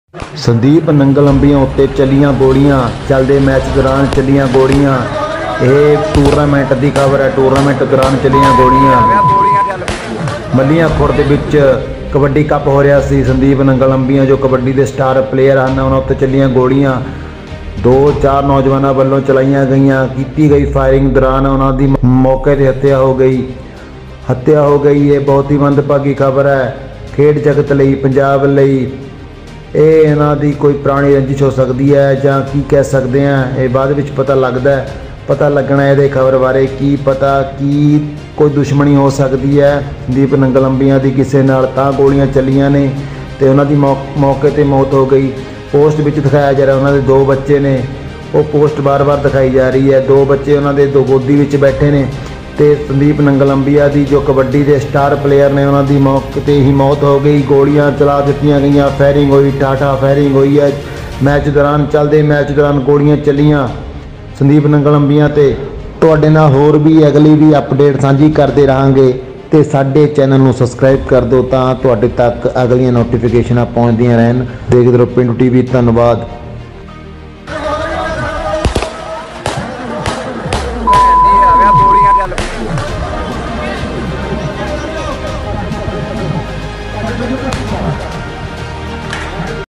Sandeep nangal ambian death | kabbadi player | pendu tv
ਸੰਦੀਪ ਨੰਗਲ ਅੰਬੀਆਂ ਤੇ ਚੱਲਦੇ ਮੈਚ ਦੋ
ਦੌਰਾਨ ਚੱਲੀਆਂ ਗੋਲੀਆਂ

kabbadi
kabbadi players
kabbadi player death
kabbadi player Punjab
kabbadi world cup
kabbadi premere lenge
sandeep nagal ambian

pendu tv